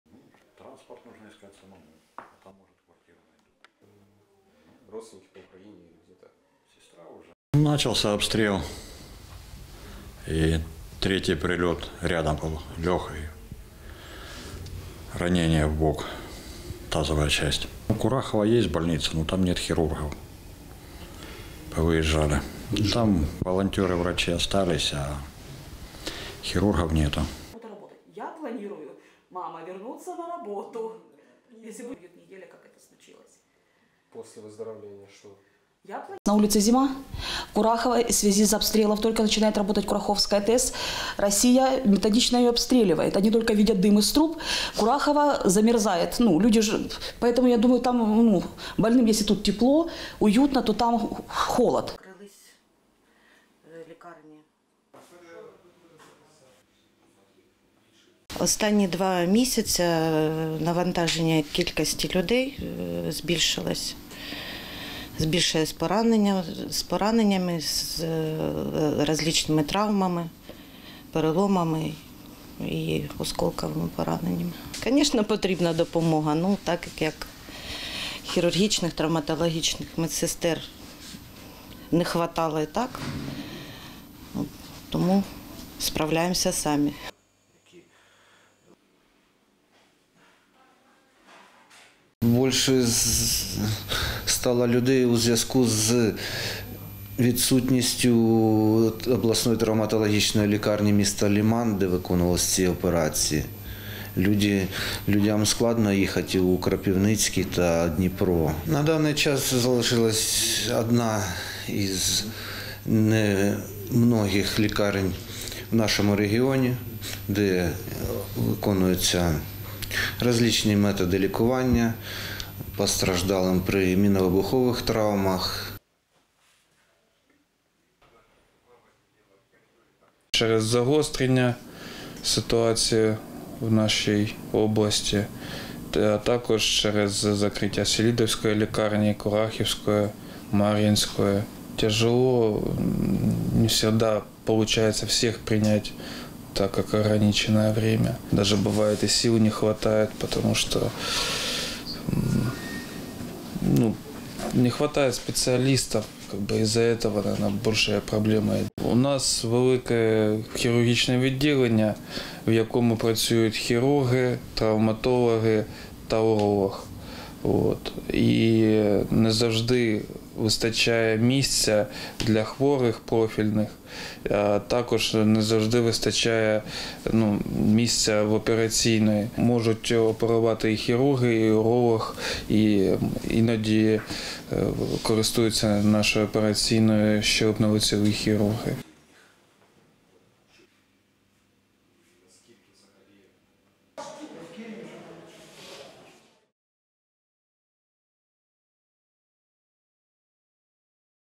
"Залишилась одна з небагатьох". Як працює клінічна лікарня у Покровську, яка приймає жителів громад
Клінічна лікарня Покровська, що на Донеччині, приймає на лікування жителів сусідніх громад. Курахівська, селидівська, мар'їнська частково або повністю не працюють. Чи справляється з таким навантаженням персонал медзакладу — у матеріалі.

Автор: Данило Черниченко
Режисер монтажу: Олександр Батарчук
Оператор: Євген Єжов